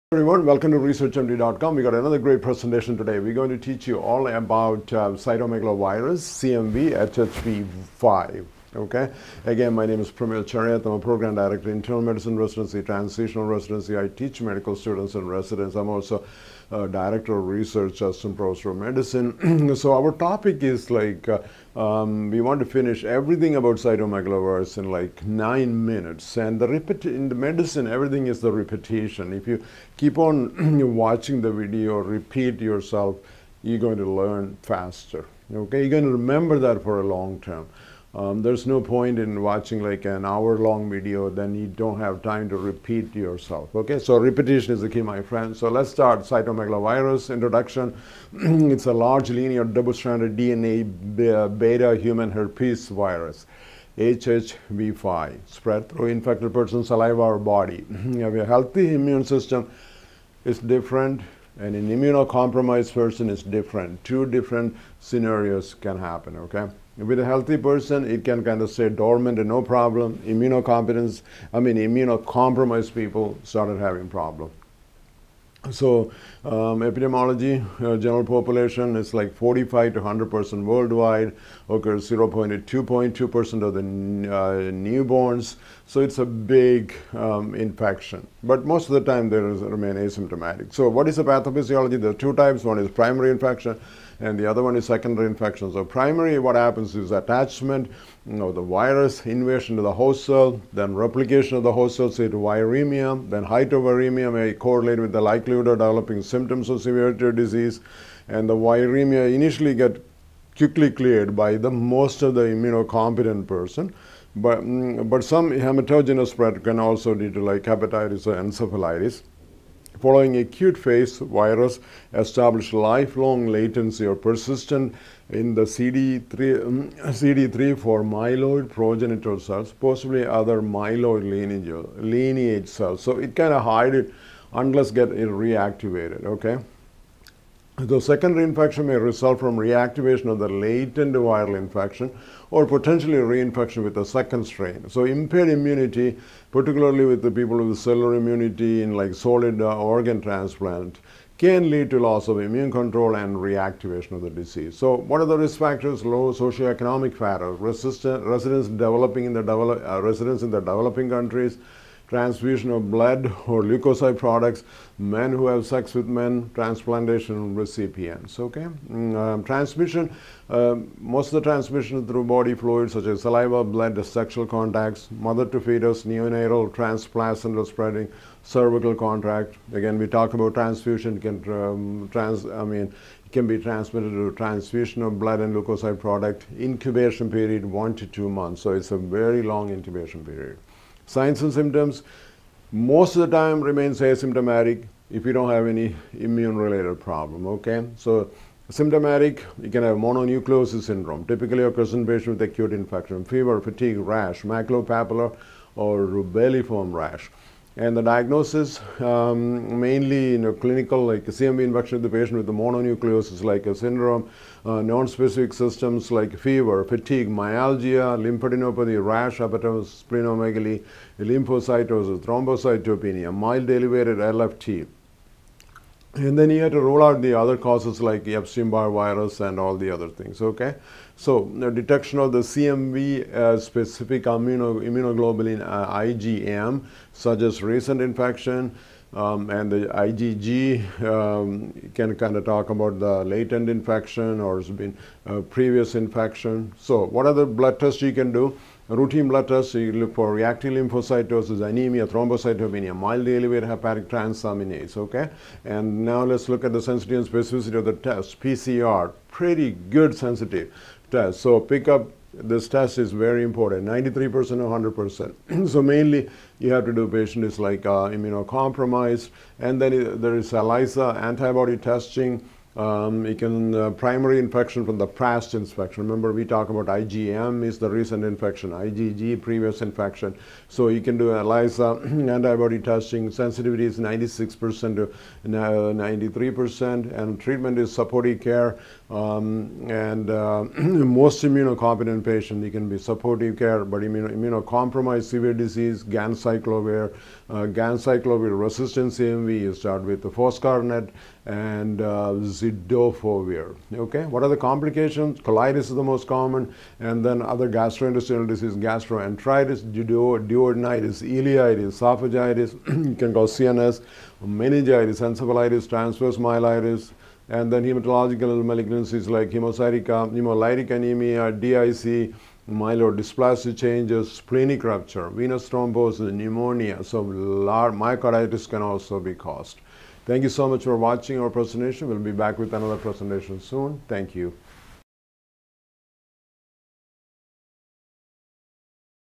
Cytomegalovirus (CMV) or HHV-5 - Explained under 9 Minutes - By Pramil Cheriyath MD
This video is not for medical advice. It is very important to consult your physician for your medical problems.
Please consult with your physician if you have any medical problems.